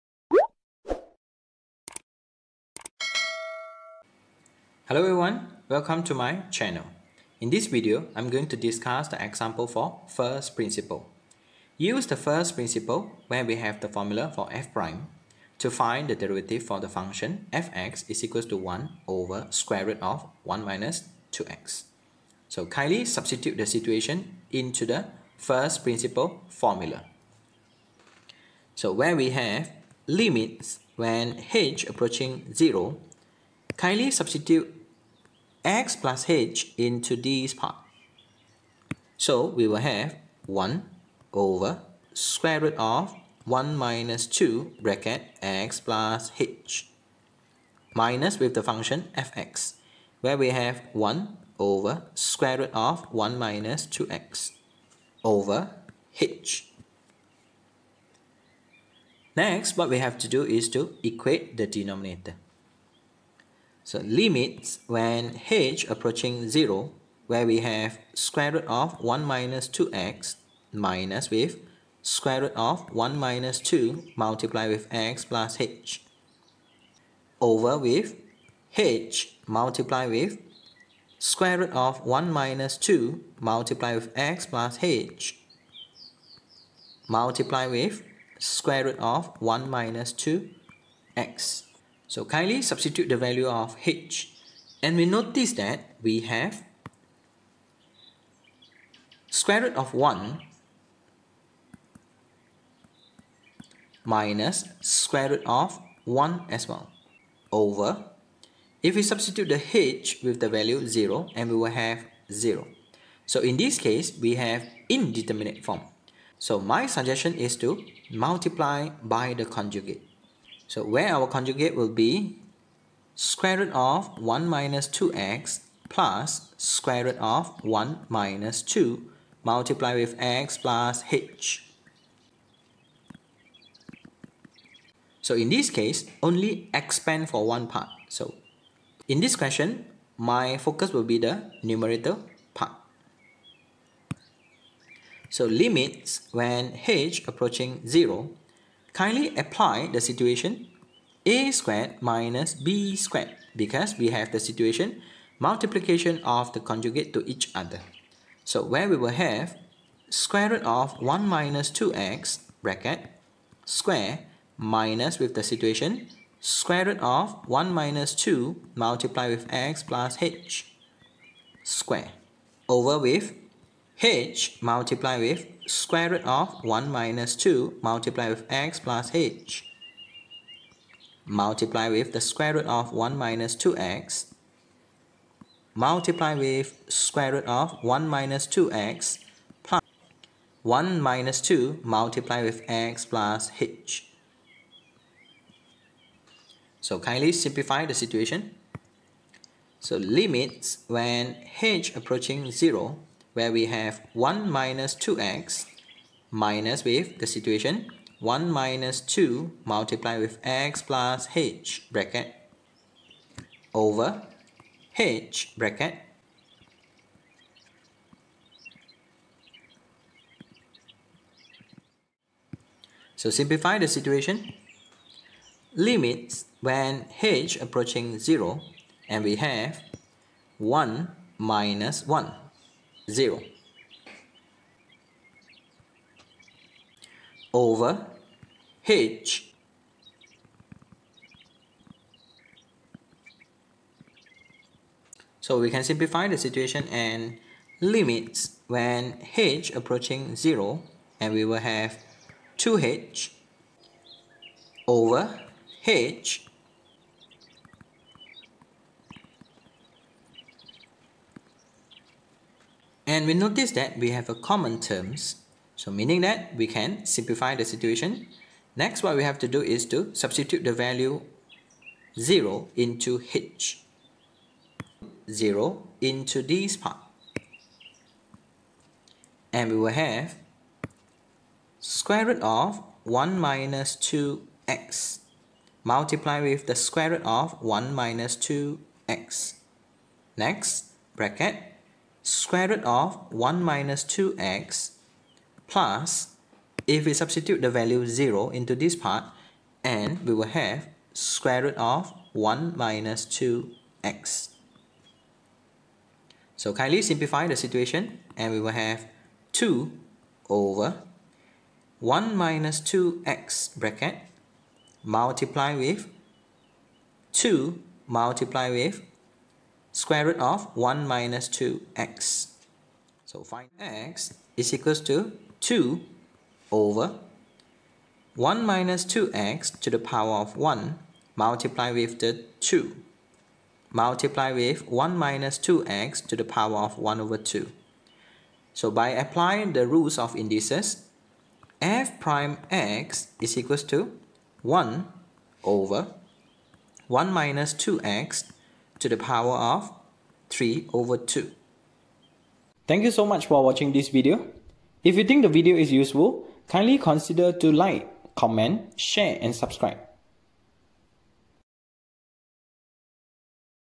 First Principle SM015 SET6 E2(b) Section A - PSPM I
In this video, we are going to discussion the question about calculus - First Principle for First Derivative

1. How to solve and deal with calculus First Principle - First Derivative